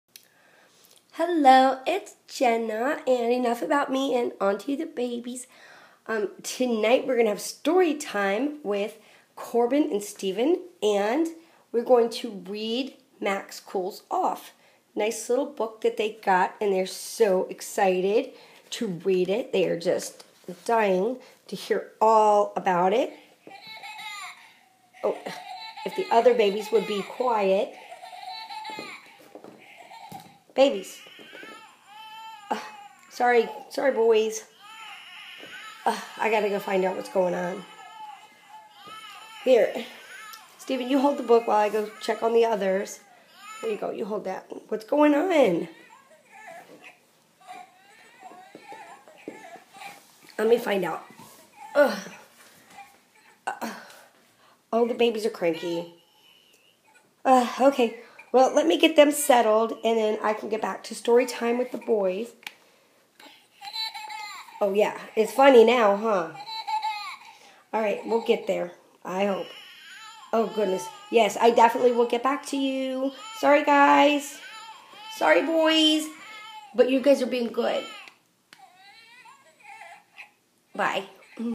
Reborn Baby Story Time Interrupted!!
I just thought I'd have a little fun with this one.  The boys were supposed to get a story, but the fussy babies interrupted everything!